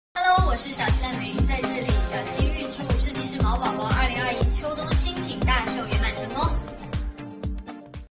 CHICCOMAO 赖美云 中国国际时装周
毛宝宝×中国国际时装周
CHICCO MAO2021秋冬新品大秀
歌手赖美云 为设计师好友送上祝福 3月27日晚8：30分不见不散[心]
中国国际时装周 CHICCOMAO工作室的微博视频 ​